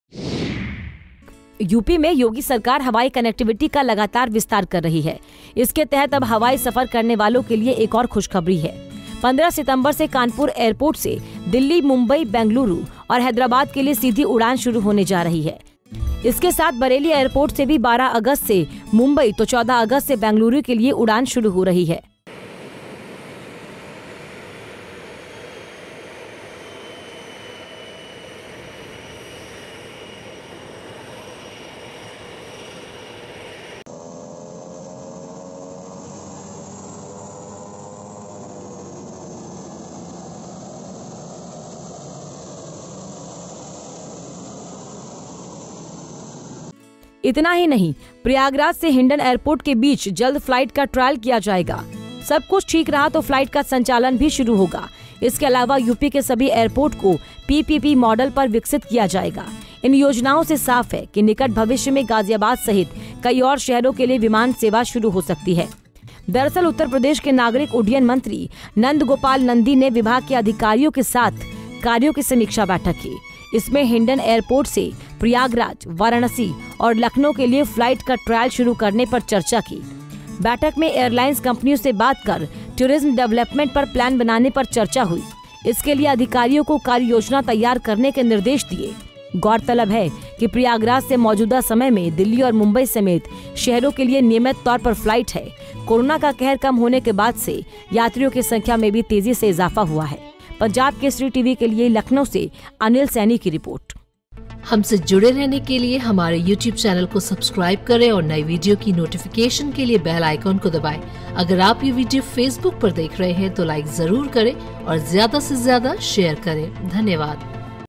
खुशखबरी: कानपुर से 15 सितंबर से शुरू होगी दिल्ली-मुंबई समेत कई बड़े शहरों के लिए सीधी फ्लाइट
यूपी में योगी सरकार हवाई कनेक्टिविटी का लगातार विस्तार कर रही है. इसके तहत अब हवाई सफर करने वालों के लिए एक और खुशखबरी है 15 सितंबर से कानपुर एयरपोर्ट से दिल्ली, मुंबई, बेंगलुरू और हैदराबाद के लिए सीधी उड़ान शुरू होने जा रही है...इसके साथ बरेली एयरपोर्ट से 12 अगस्त से मुंबई तो 14 अगस्त से बेंगलुरू के लिए उड़ान शुरू हो रही है...इसके साथ बरेली एयरपोर्ट से भी 12 अगस्त से मुंबई तो 14 अगस्त से बेंगलुरू के लिए उड़ान शुरू हो रही है. इतना ही नहीं प्रयागराज से हिंडन एयरपोर्ट के बीच जल्द फ्लाइट का ट्रायल किया जाएगा...